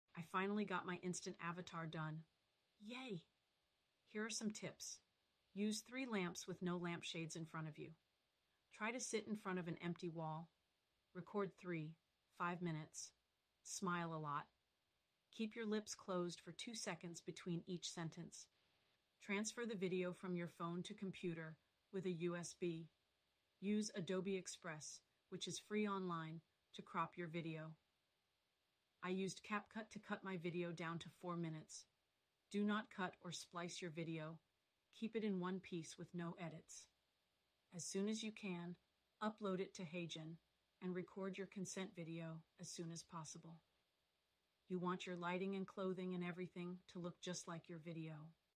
I made my Avatar in HeyGen!
I finally got my Instant Avatar done. Yay! Here are some tips:
Use 3 lamps with no lampshades in front of you. Try to sit in front of an empty wall. Record 3 - 5 minutes. Smile a lot. Keep your lips closed for 2 seconds between each sentence. Transfer the video from your phone to computer with a USB. Use Adobe Express, which is free online, to crop your video. I used Capcut to cut my video down to 4 minutes. Do not cut or splice your video, keep it in one piece with no edits.
As soon as you can, upload it to Heygen, and record your consent video as soon as possible. You want your lighting and clothing and everything to look just like your video.

If you want to use the "YouCam Video" app or the "Facetune" apps on your phone to touch up your video, wait until after you get your raw video  and your consent recording approved.

I wanted to touch mine up before so I didn't have to do it with ever video, but that didn't work. My consent video failed.

If you have an iPhone and a PC, the only way to get your video from your PC to your iPhone is to email it, and open your email on your phone and save it.

You can, however, use a USB cable to transfer your video from you iPhone to your PC after you touch it up with the phone apps.

FaceTune was better for touching up flaws in your skin, and increasing your volume. YouCam Video was much better at makeup.

You can save your settings for your touch ups and makeup on these apps, and apply them to new videos, to keep your look consistent.